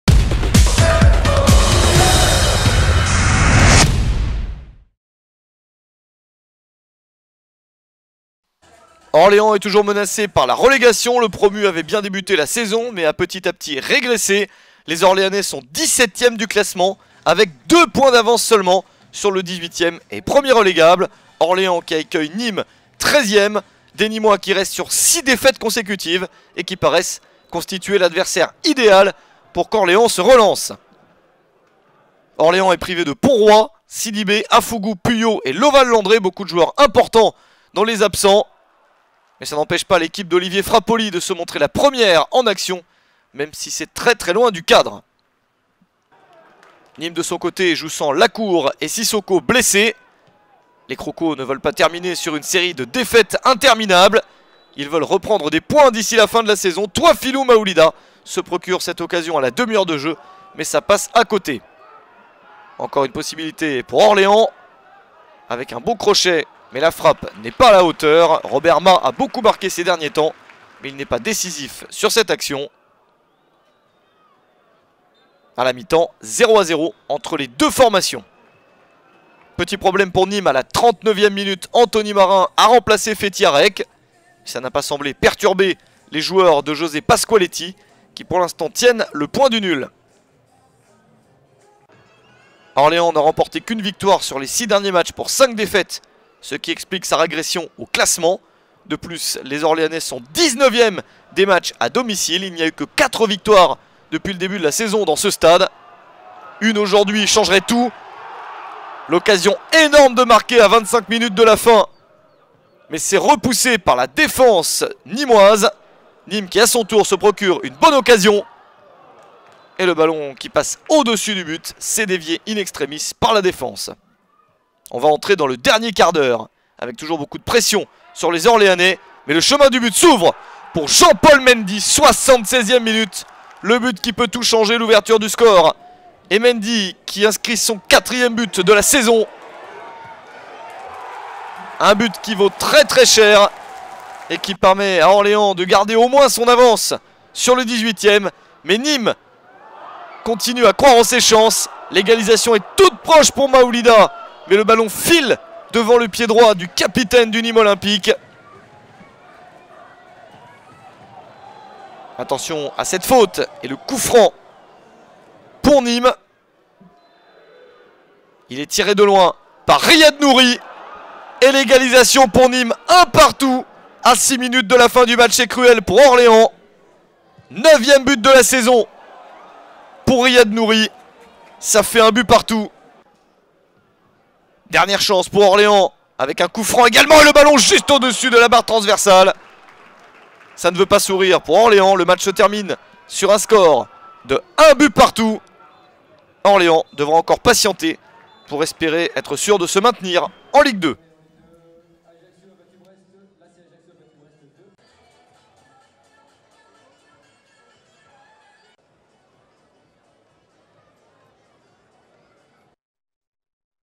US Orléans - Nîmes Olympique (1-1) - Résumé - (USO - NIMES) / 2014-15
Revivez les meilleurs moments de US Orléans - Nîmes Olympique (1-1) en vidéo.

 - vendredi 8 mai 2015

Buteurs : Jean-Paul MENDY (76' - US Orléans) / Riad NOURI (84' - Nîmes Olympique)

Composition US Orléans : 3 - Gregory TOMAS / 8 - Jean-Paul MENDY / 16 - Arnaud BALIJON / 17 - Luigi GLOMBARD / 18 - Robert MAAH / 19 - Bruce ABDOULAYE / 20 - Matthieu LIGOULE / 22 - Gauthier PINAUD / 23 - Julien DELONGLEE / 24 - Samir BENMEZIANE / 25 - Maxime BRILLAULT (c)

Composition Nîmes Olympique : 1 - Mathieu MICHEL / 2 - Jérémy CORDOVAL / 6 - Jonathan PARPEIX / 7 - Mathieu ROBAIL / 8 - Toifilou MAOULIDA (c) / 12 - Romain ELIE / 13 - Panagiotis VLACHODIMOS / 17 - Fethi HAREK / 21 - Riad NOURI / 25 - Anthony KOURA / 29 - Fabien BARRILLON